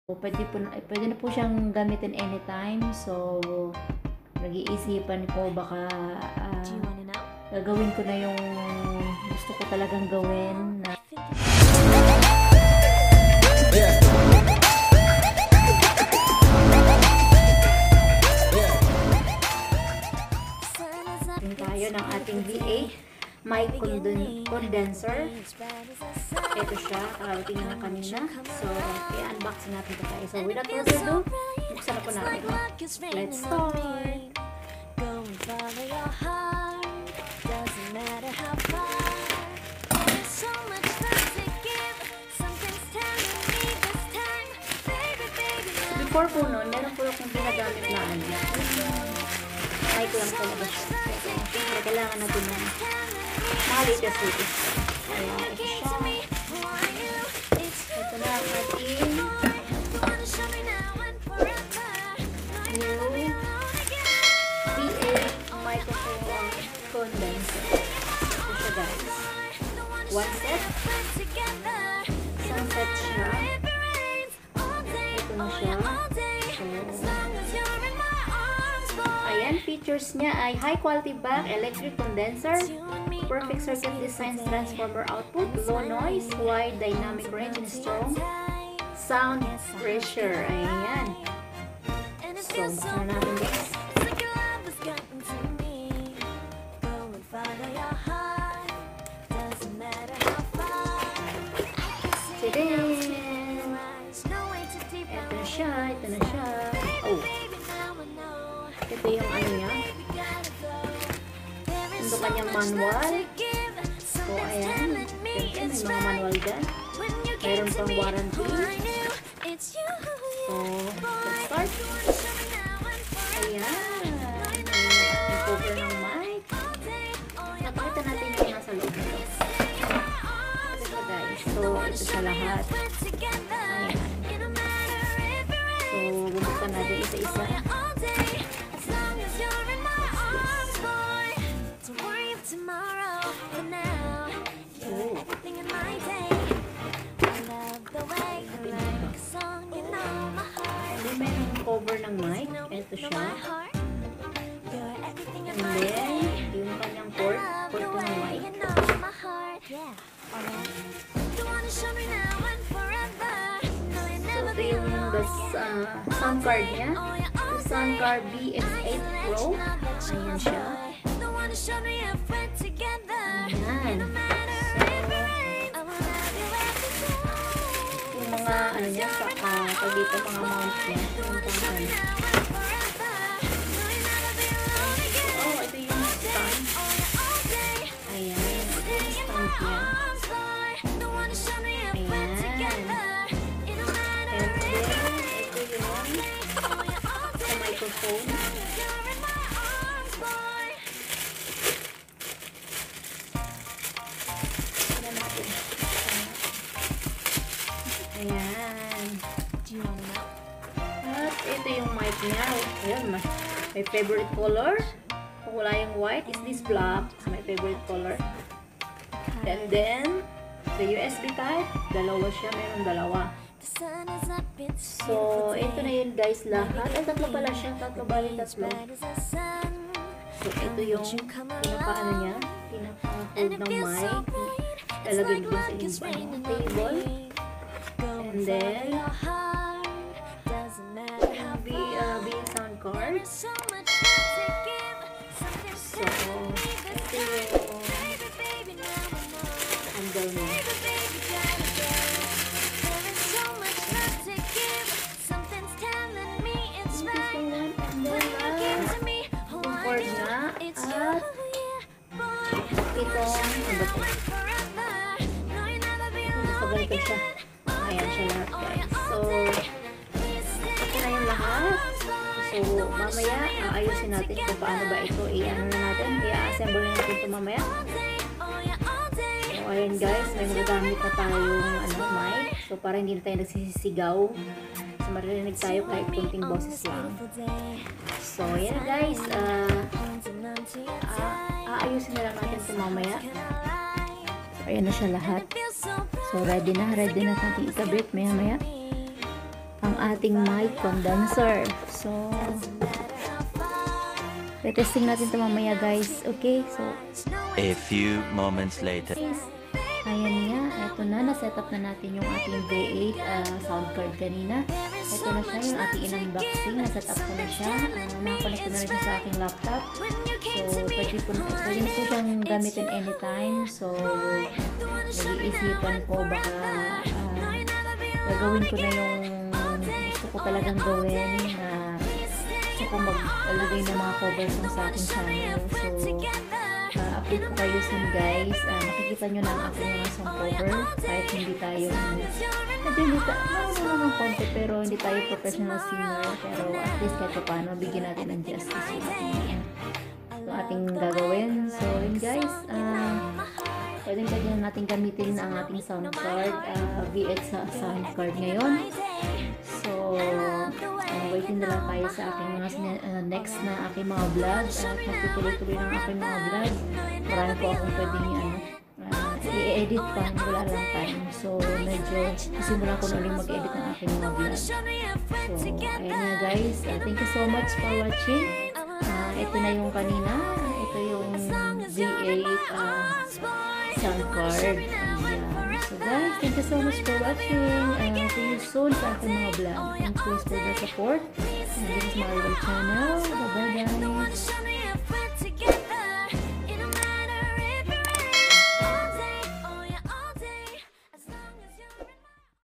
Unboxing Microphone Condenser Kit | BM-800 Microphone | Sound Card V8X pro | MariVlog's Channel
This video is about unboxing my new BM-800 Microphone + Sound Card V8X pro